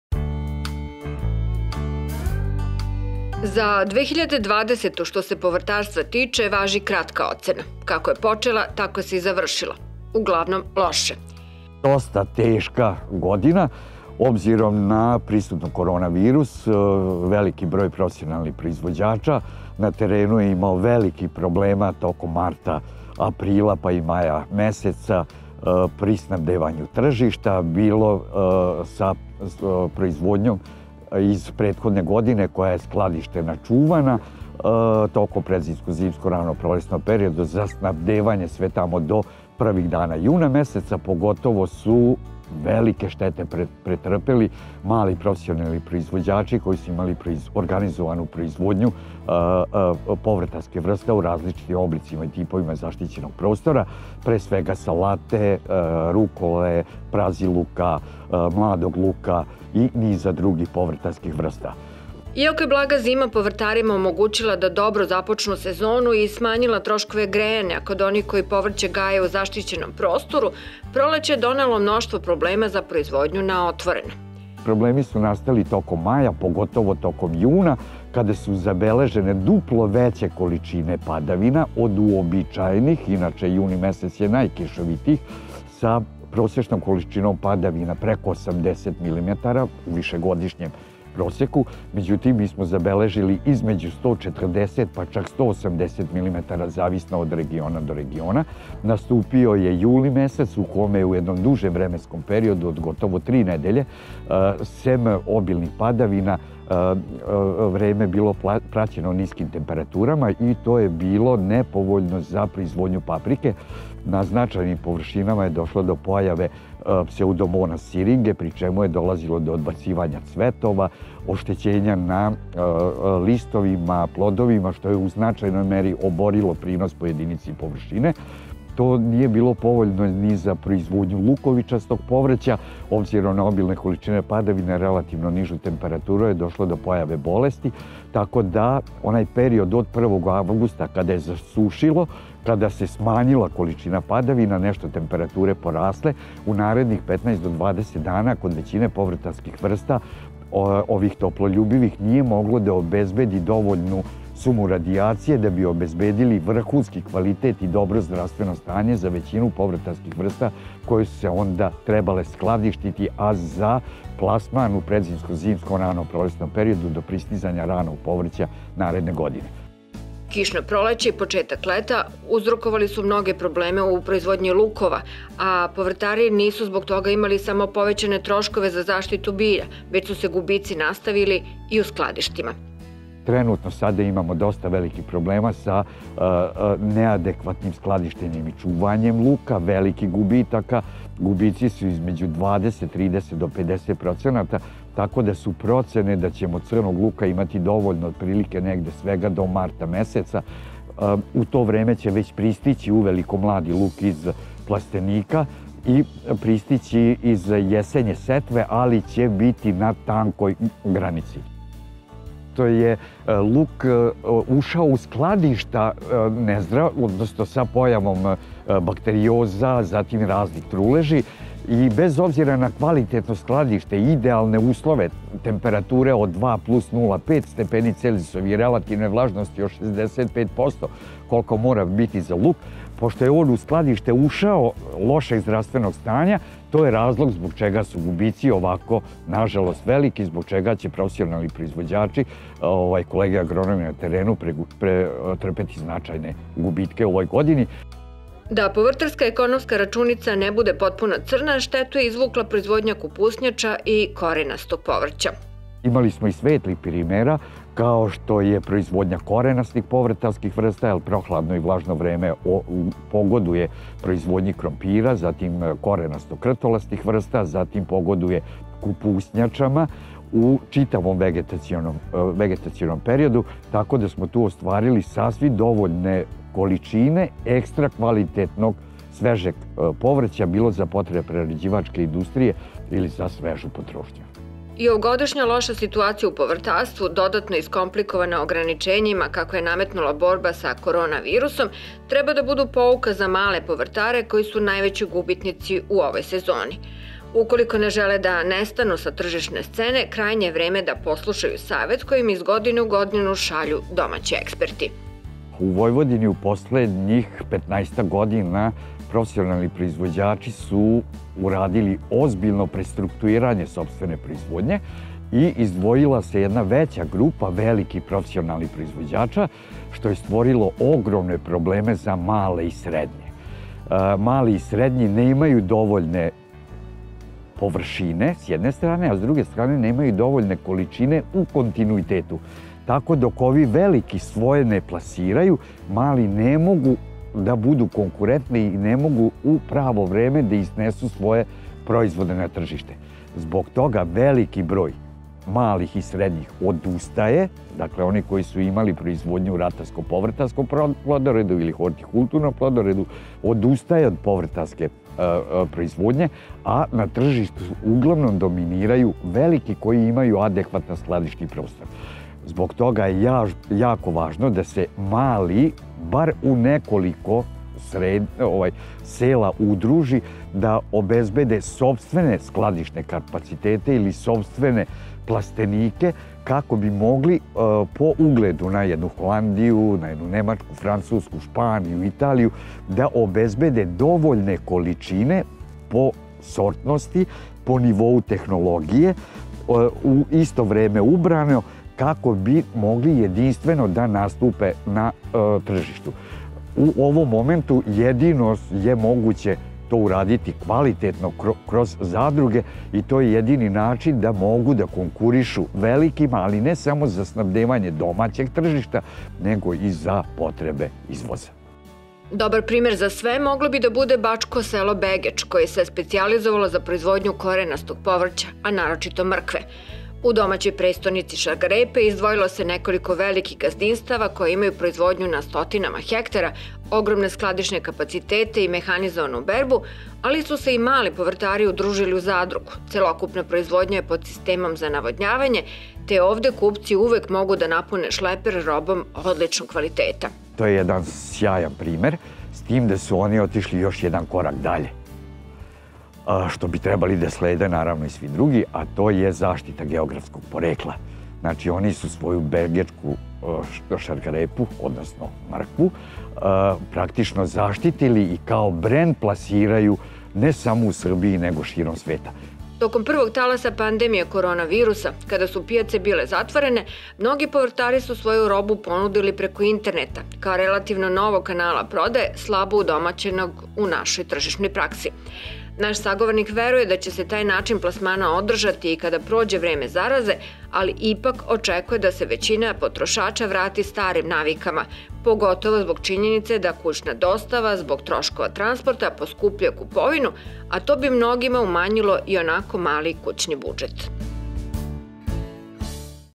CRNA SEZONA za povrtarstvo
Zašto su mali proizvođači povrća najveći gubitnici u ovoj poljoprivrednoj sezoni?
Sagovornik: Prof. dr Žarko Ilin –Poljoprivredni fakultet - Novi Sad
Facebook : Farma emisija o poljoprivredi